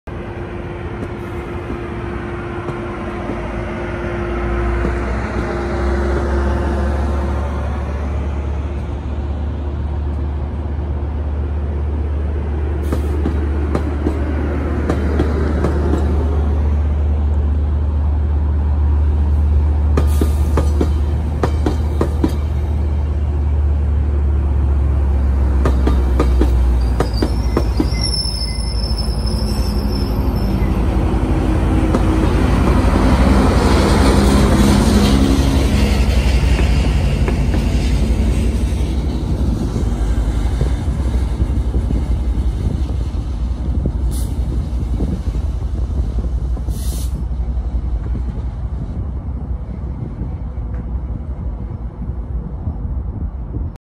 Metrolink F59PHI 884 pushing out of Riverside en route to Los Angeles Union Station, California
Metrolink F59PHI 884 pushing out of Riverside-Downtown Station en route to Los Angeles Union Station, California.